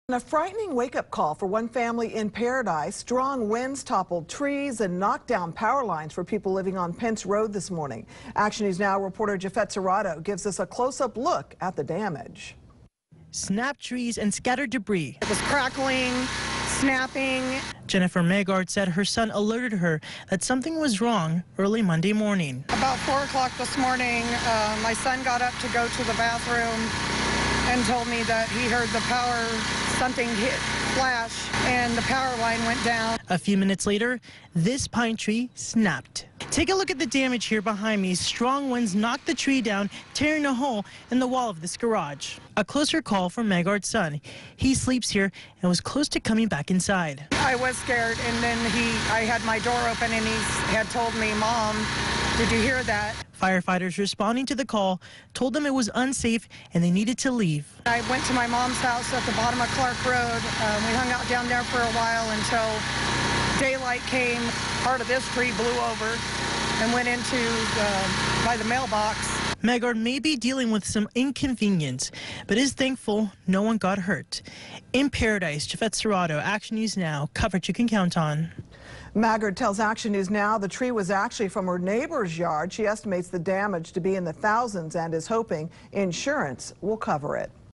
Strong winds topple tree onto Paradise family's garage
A frightening wake-up call for one family in paradise. Strong winds and toppling trees early Monday caused a lot of damage to their property.